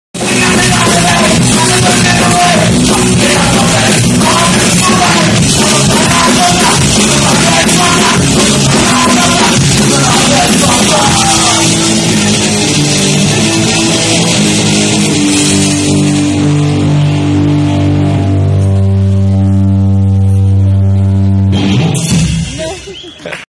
Nájezd Trolů - kino_HD_2019
Proběhl další úspěšný nájezd spojený s obsazením nové starodávné lidské osady jménem kino. Bylo nám ctí státi v prvních řadách našich spolubojovníků a zaútočit skoro v plné síle, mnoho padlo a mnoho ješte padne. Nechtˇ je totu území považováno za podrobené!